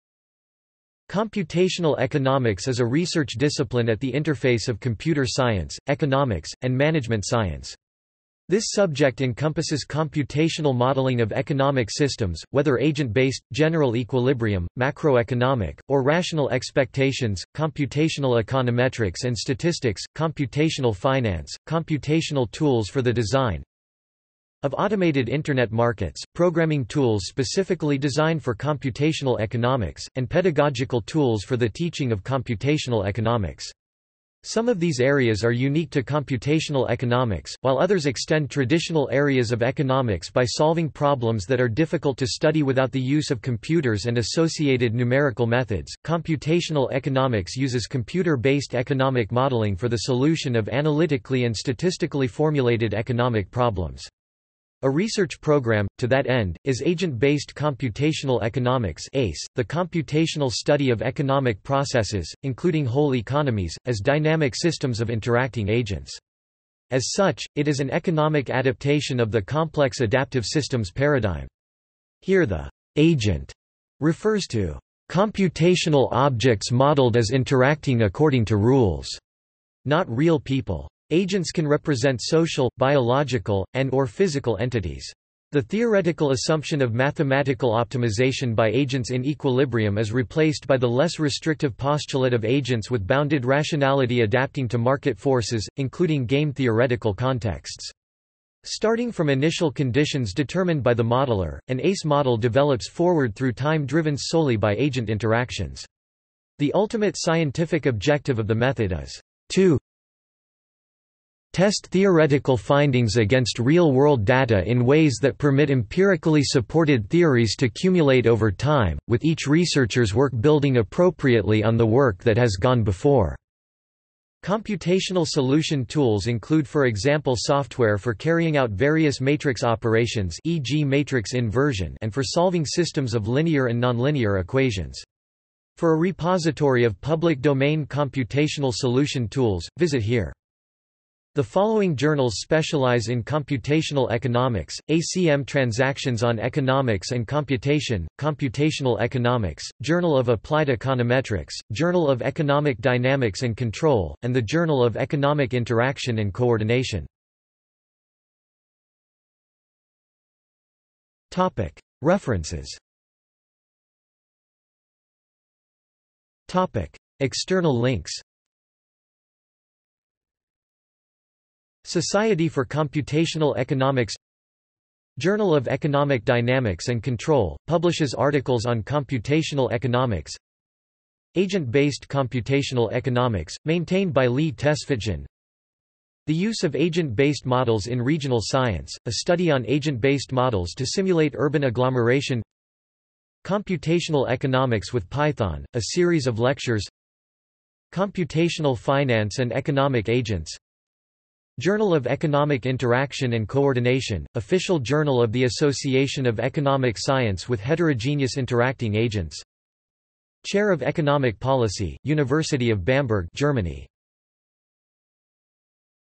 Computational economics | Wikipedia audio article
This is an audio version of the Wikipedia Article:
Computational economics

- Socrates

SUMMARY
Computational economics is a research discipline at the interface of computer science, economics, and management science. This subject encompasses  computational modeling of economic systems, whether agent-based, general-equilibrium,  macroeconomic, or rational-expectations, computational econometrics and statistics, computational finance, computational tools for the design
of automated internet markets, programming tools specifically designed for computational economics, and pedagogical tools for the teaching of computational economics.  Some of these areas are unique to computational economics, while others extend traditional areas of economics by solving problems that are difficult to study without the use of computers and associated numerical methods.Computational economics uses computer-based economic modeling for the solution of analytically and statistically formulated economic problems. A research program, to that end, is agent-based computational economics (ACE), the computational study of economic processes, including whole economies, as dynamic systems of interacting agents. As such, it is an economic adaptation of the complex adaptive systems paradigm.  Here the "agent" refers to "computational objects modeled as interacting according to rules," not real people. Agents can represent social, biological, and/or physical entities. The theoretical assumption of mathematical optimization by agents in equilibrium is replaced by the less restrictive postulate of agents with bounded rationality adapting to market forces, including game-theoretical contexts. Starting from initial conditions determined by the modeler, an ACE model develops forward through time driven solely by agent interactions. The ultimate scientific objective of the method is "to ... test theoretical findings against real-world data in ways that permit empirically supported theories to cumulate over time, with each researcher’s work building appropriately on the work that has gone before."Computational solution tools include for example software for carrying out various matrix operations (e.g. matrix inversion) and for solving systems of linear and nonlinear equations. For a repository of public-domain computational solution tools, visit  here.
The following journals specialize in computational economics: ACM Transactions on Economics and Computation, Computational Economics, Journal of Applied Econometrics, Journal of Economic Dynamics and Control, and the Journal of Economic Interaction and Coordination.